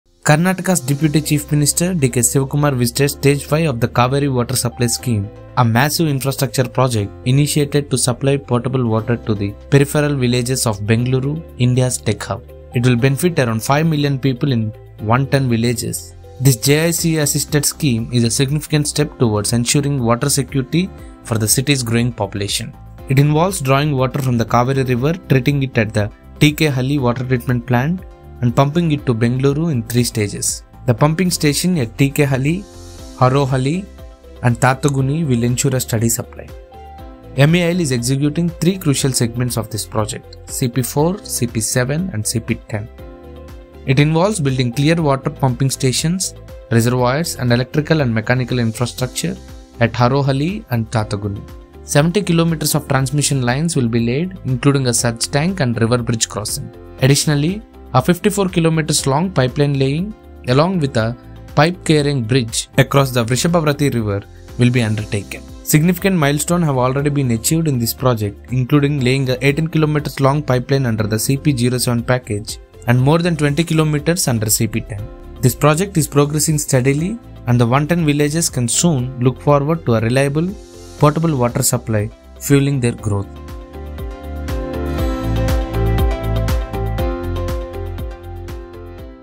Karnataka's Deputy CM Visits Cauvery Water Supply Scheme | Megha Engineering
Karnataka's Deputy Chief Minister DK Shivakumar visited Stage 5 of the Cauvery Water Supply Scheme, a project aimed at providing clean drinking water to 110 peripheral villages of Bengaluru. This initiative, supported by the Japan International Cooperation Agency (JICA), involves drawing water from the Cauvery River, treating it at the T K Halli Water Treatment Plant, and pumping it in three stages. MEIL is executing three crucial segments of the project, where significant milestones have already been achieved, and this project is expected to bring reliable potable water supply to the villages soon.